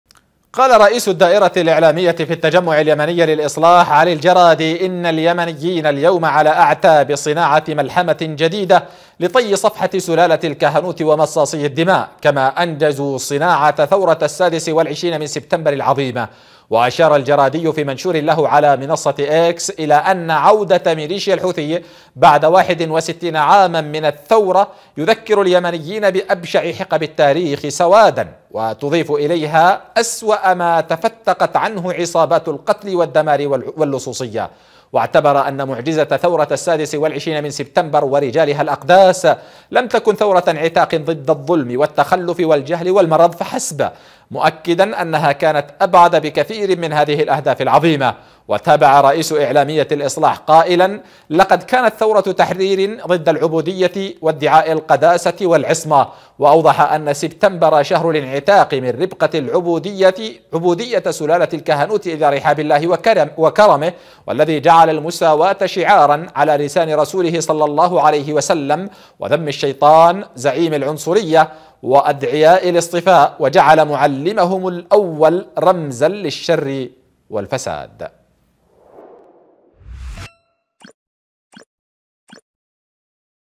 رئيس الدائرة الإعلامية للإصلاح : 26 سبتمبر ثورة تحرر واليمنيون على أعتاب صناعة ملحمة جديدة تطوي صفح
قناة_سهيل# ‪